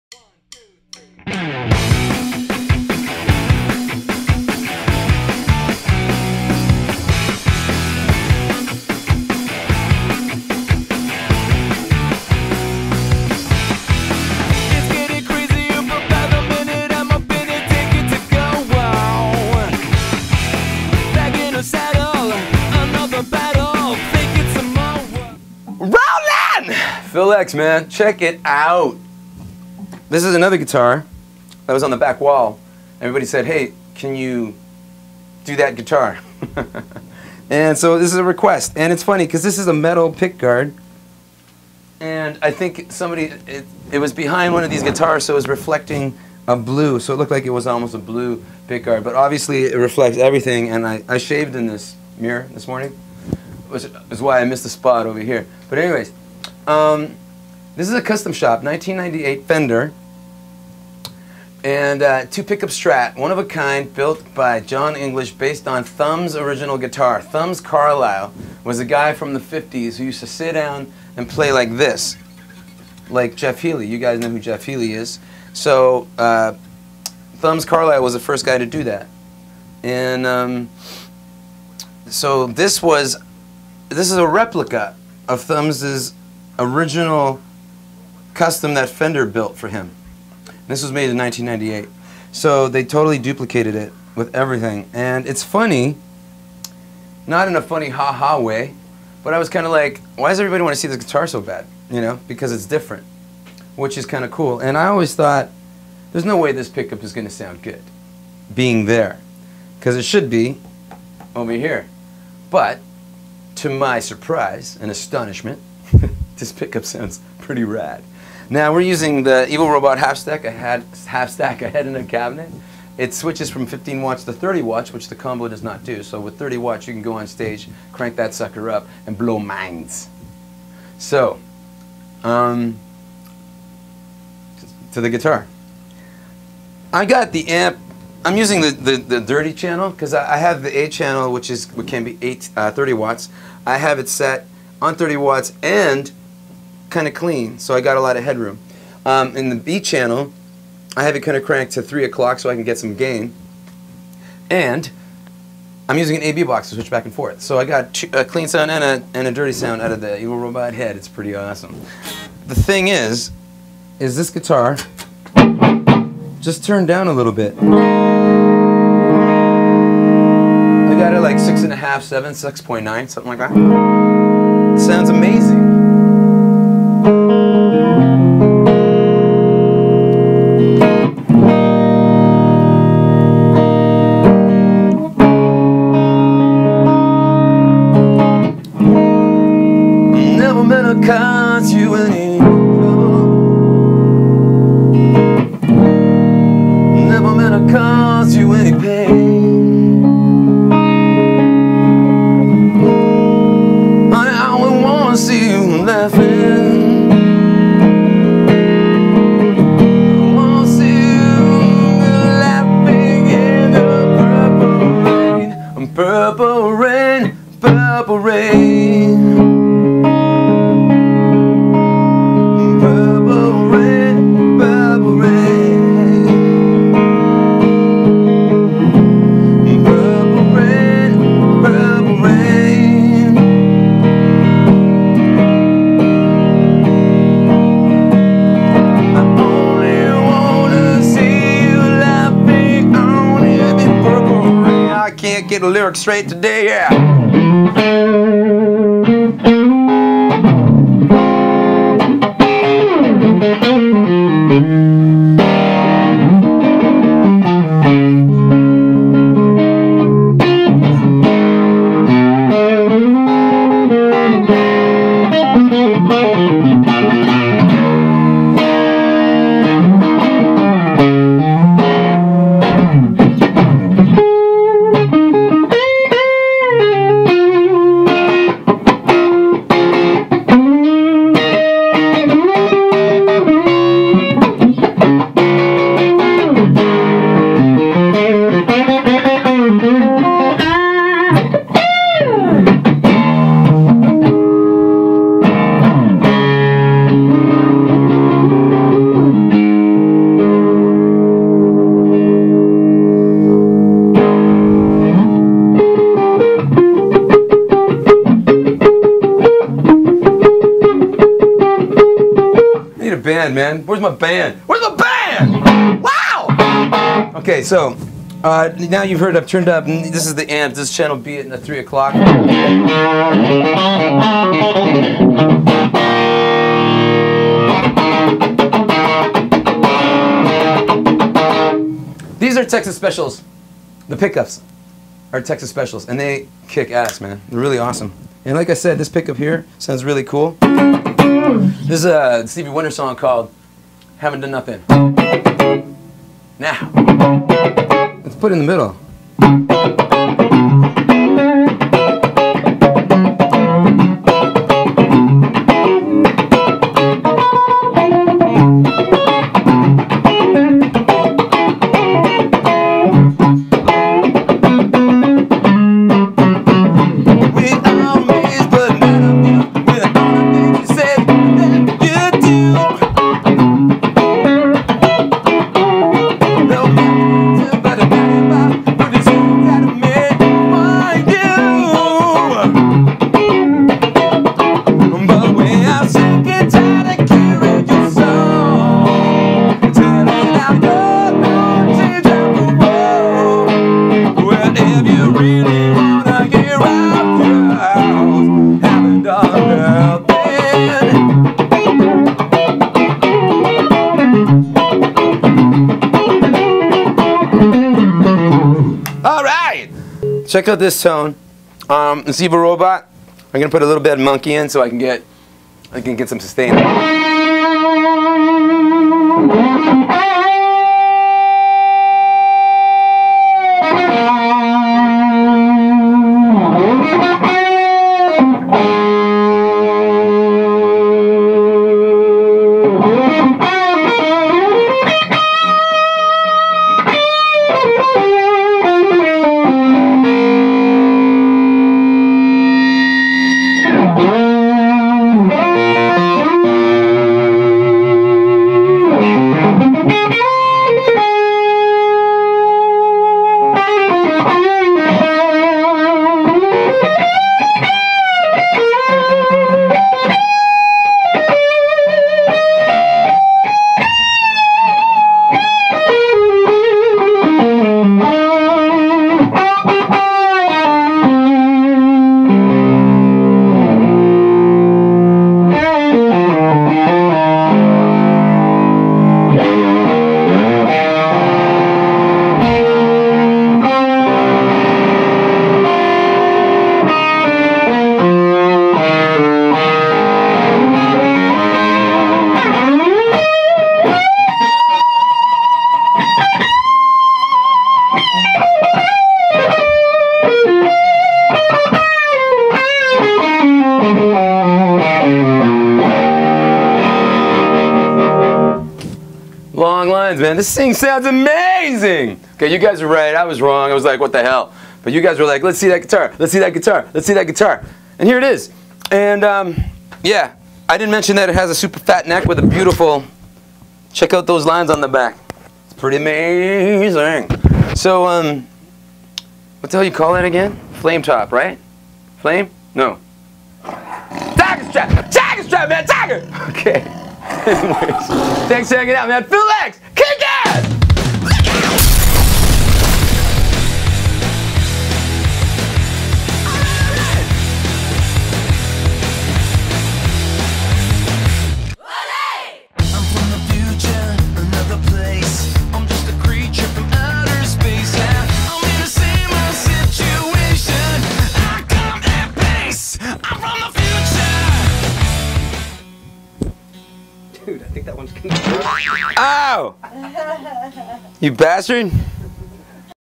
PHIL X 1998 Thumbs Strat 01002
1955 Fender Stratocaster, two pickup model. 1998 Custom Shop prototype, stripped to natural finish. This is a prototype based on the extremely rare guitar, custom built for and formerly owned by the innovative player and songwriter "Thumbs" Carllile as seen in "The Galaxy of Strats" book (pages 130-131). Back of peghead reads "Proto" Thumb's and has John's decal, includes a copy of the Letter of Authenticity from John English to then owner Larry Briggs and the original Custom Shop fax from John to Larry. Complete with center pocket Tweed case.